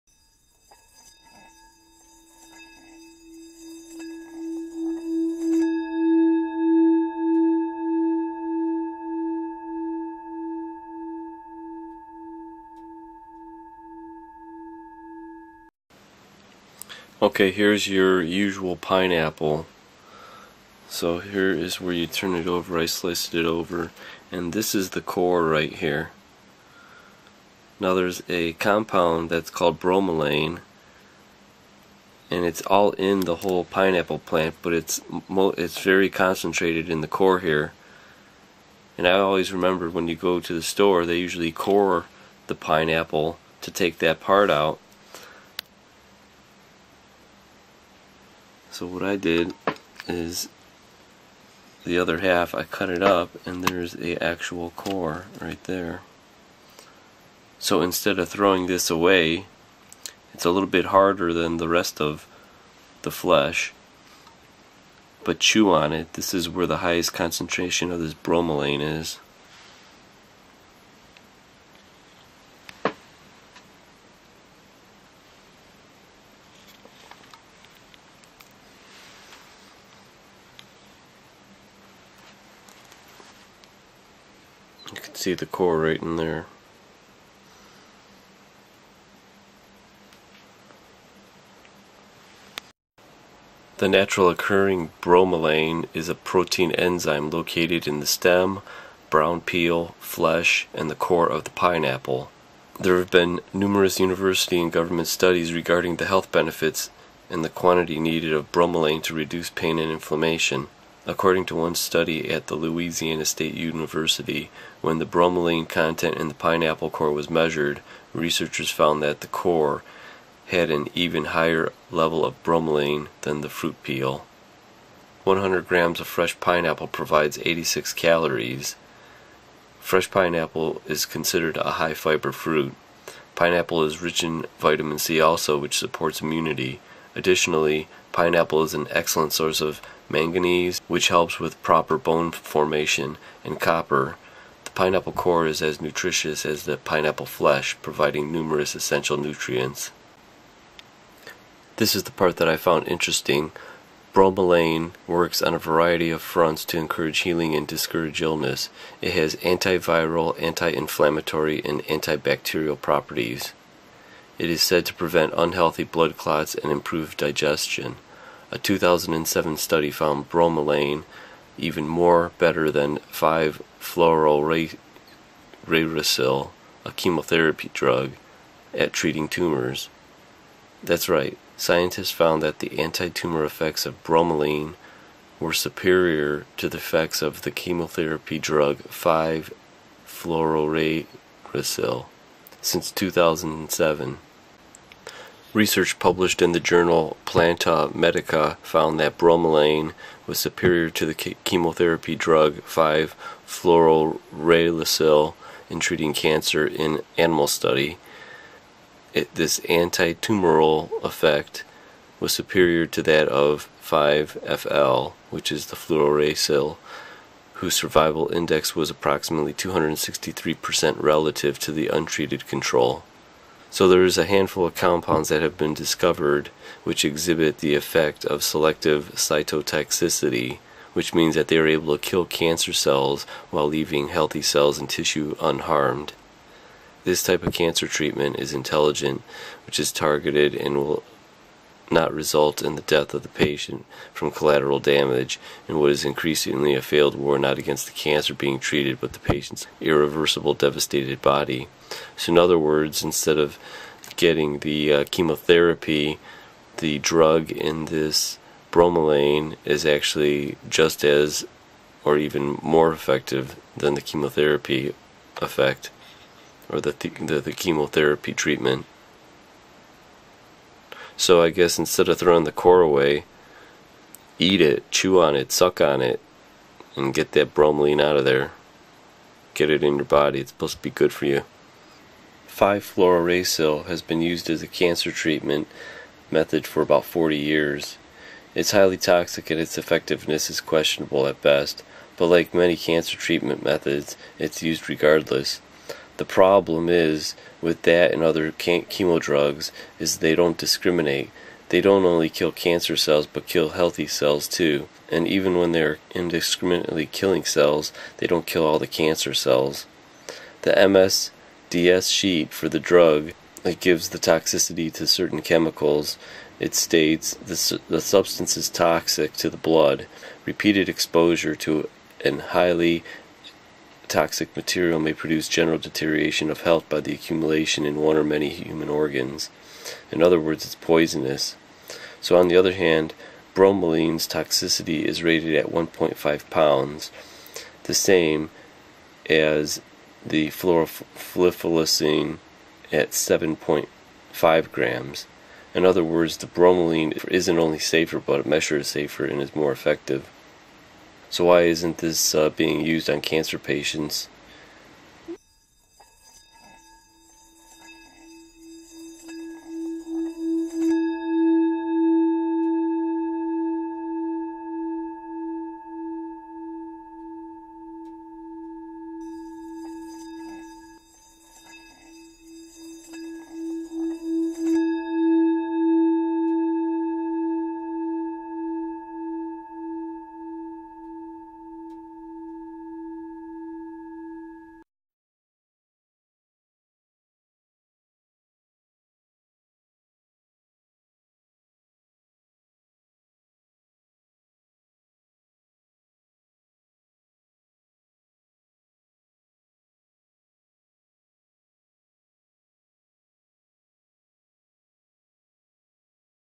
Pineapple Bromelain vs 5-Fluorouracil in treating cancer 5-14-2013 | Organic Slant
Published in 2007 in the journal Planta Medica, researchers found that an enzyme extracted from pineapple stems known as bromelain was superior to the chemo-agent 5-fluorauracil in treating cancer in the animal model.